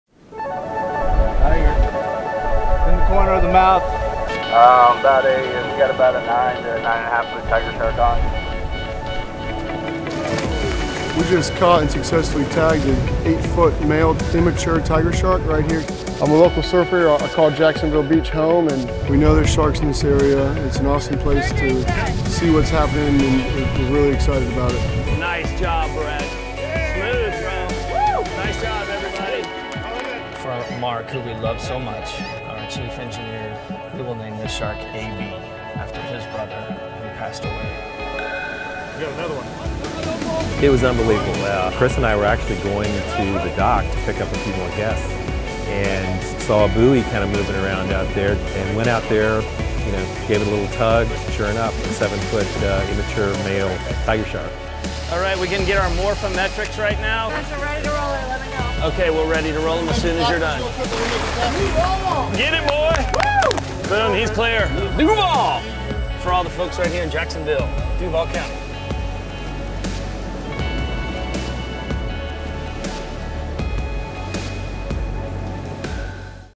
Expedition Jacksonville: Tagging AB & Duval
The tiger sharks keep showing up behind the boat! Two more tiger shark are tagged, sampled and released by our collaborative science team. Watch the tagging of AB and Duval! ExpeditionJax 2016.